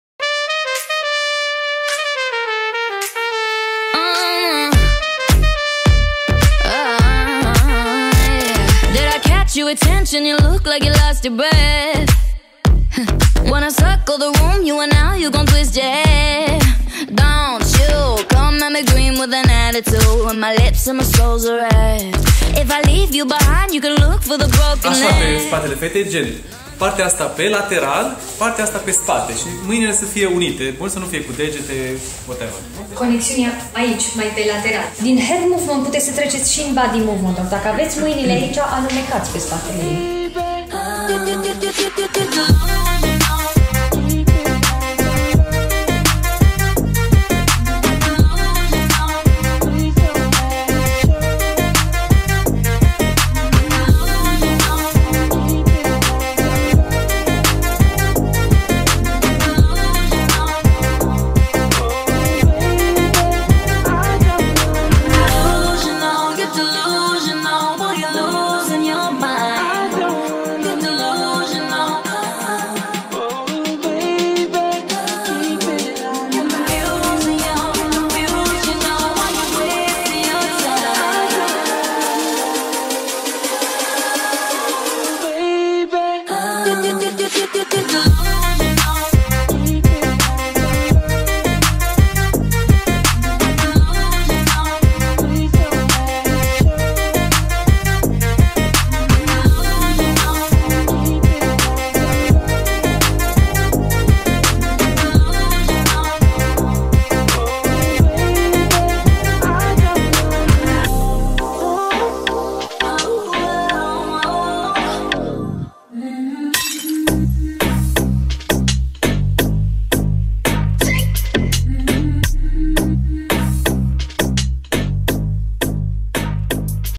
Zouk Dance Bootcamp After movie 2021 Romania
Made by Alex Popovici, filmed with mcpro24fps, powered by AMVR mods^^ on LG G8 and G7 with ShiftCam 12mm conversion Lens